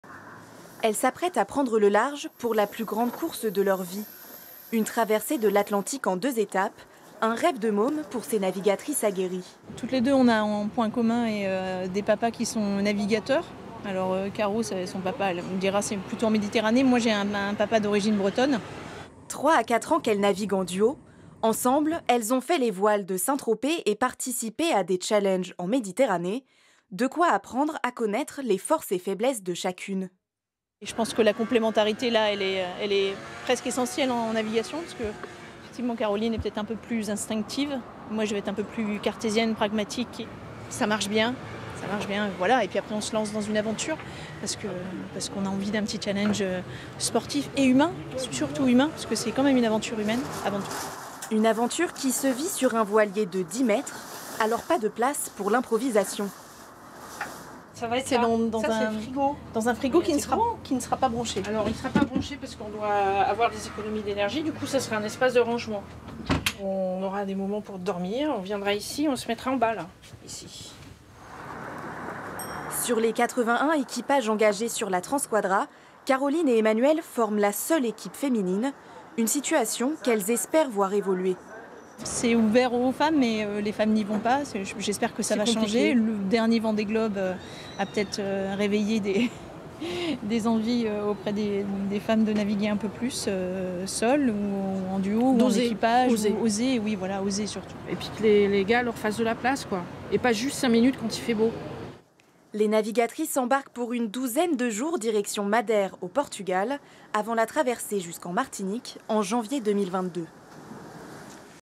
Marseille : 10e édition de la Transquadra, le seule duo féminin engagé
Emmanuelle Blivet et Caroline Petit, Cheffes d'entreprise et navigatrices participent pour la première fois à la Transquadra, une course au large ouverte à tous les marins amateurs de plus de 40 ans.  C'est la 10ème édition, mais le premier départ de Marseille car le port d'attache reste toujours Lorient.